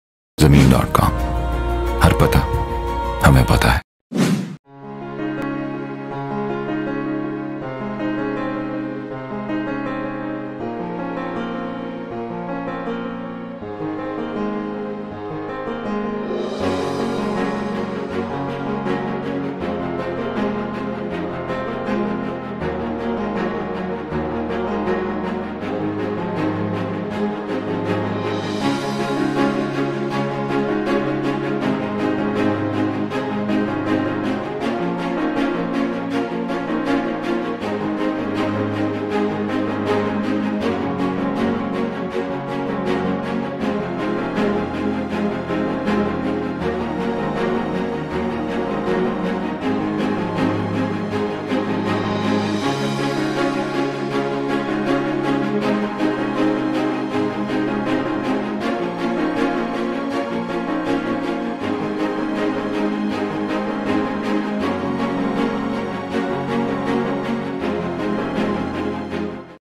1 KANAL PLOT FILE FOR SALE IN DHA MULTAN
1 Kanal Plot File Available
Ideal For investment and living purposes.

ideal Location in Multan
Largest Housing society in Southern Punjab
Includes All essentials for Livings

We are Registered Agents of DHA Multan